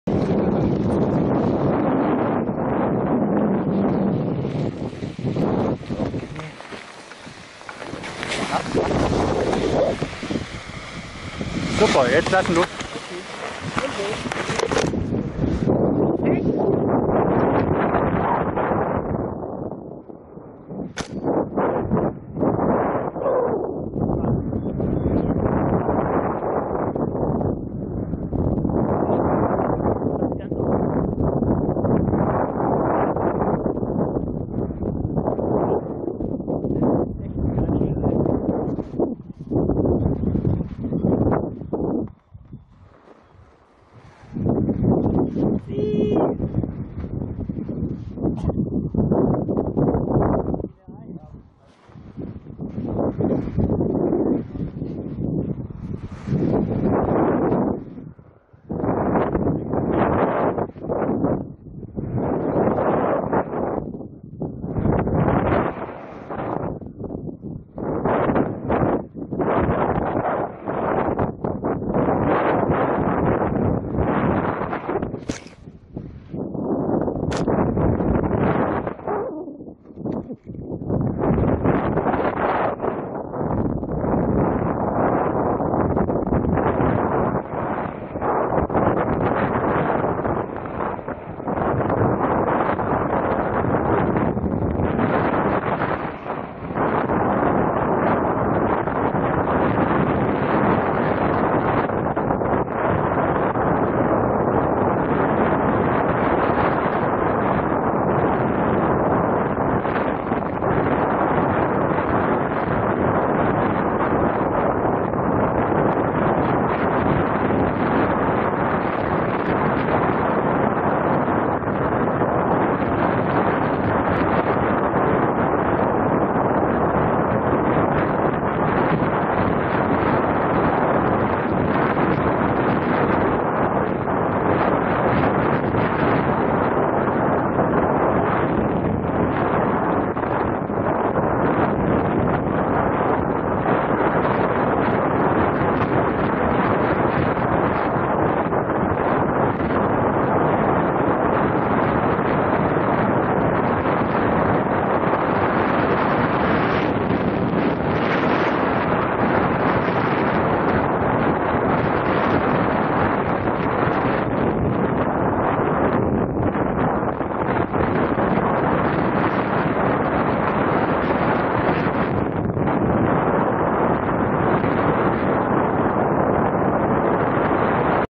Start vom Vorderunnütz nach Osten und Flug nach Norden mit Verfolgerkamera
Experimenteller Einsatz der Verfolgerkamera.

Zum Flug: Bei mittlerem Wind aus Ost-Südost bin ich am Gipfel des Vorderunnütz gestartet. Zunächst ging die Route nach Südost. Da ich den Unnütz auf diesem Weg nicht mehr südlich umfliegen konnte, ging der weitere Flug dann östlich vom Gipfel in Richtung Norden.

Zur Software: Das Video war zunächst immer noch sehr verwackelt. Mit Hilfe von ffmpeg und vid.stab konnte ich das mildern. Allerdings bleiben Verzerrungseffekte.